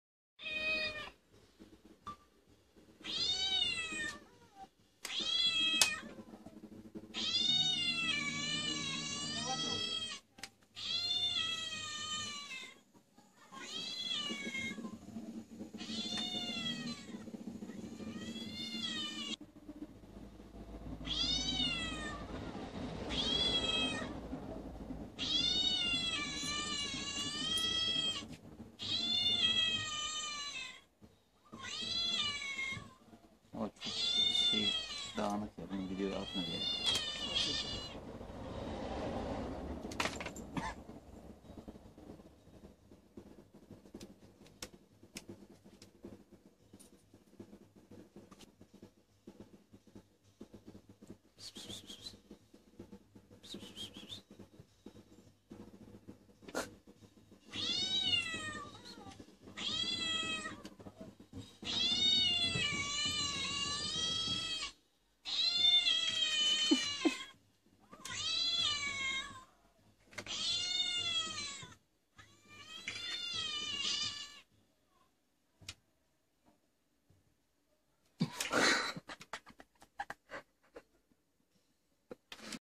Kedi Seslerini Heyecanla Arayan Karadut (Black Mulberry)
Karadut, kedi miyavlamasının nereden geldiğini ünlü bir dedektif Sherlock Holmes gibi kitaplar arasında didik didik arıyor (:

Karadut, looking at where the voice of the cat came from - like a well-known detective Sherlock Holmes - is searching between the books (: